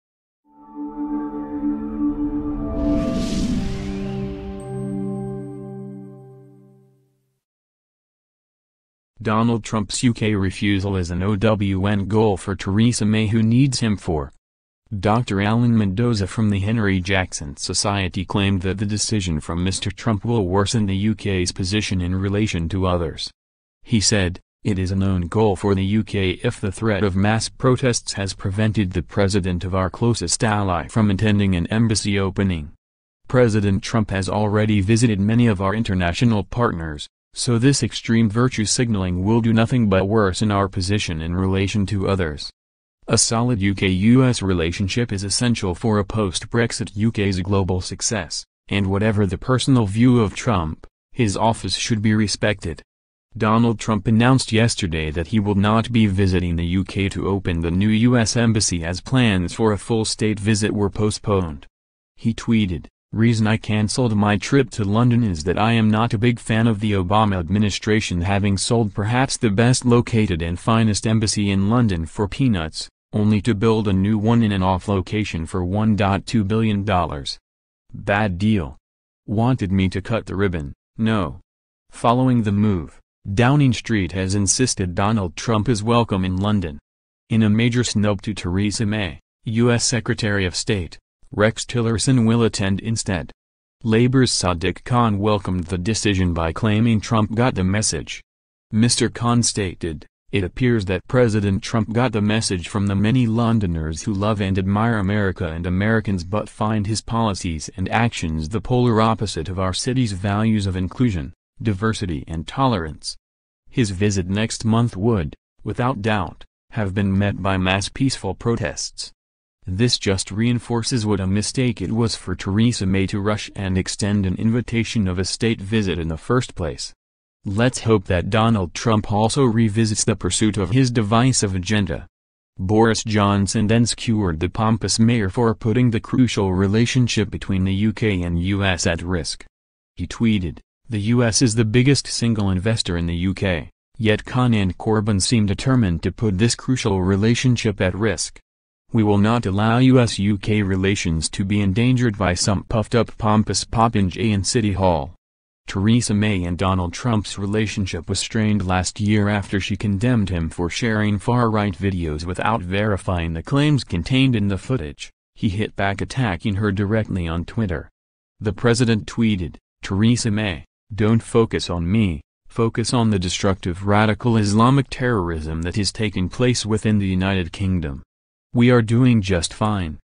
Donald Trump’s UK refusal is an ‘OWN GOAL’ for Theresa May who 'needs him for Brexit'
Donald Trump’s UK refusal is an ‘OWN GOAL’ for Theresa May who 'needs him for.
Dr Alan Mendoza from the Henry Jackson Society claimed that the decision from Mr Trump will “worsen” the UK’s position “in relation to others”.

He said: “It is an own goal for the UK if the threat of mass protests has prevented the President of our closest ally from attending an embassy opening.

“President Trump has already visited many of our international partners, so this extreme virtue signalling will do nothing but worsen our position in relation to others.

“A solid UK-US relationship is essential for a post-Brexit UK’s global success, and whatever the personal view of Trump, his office should be respected.”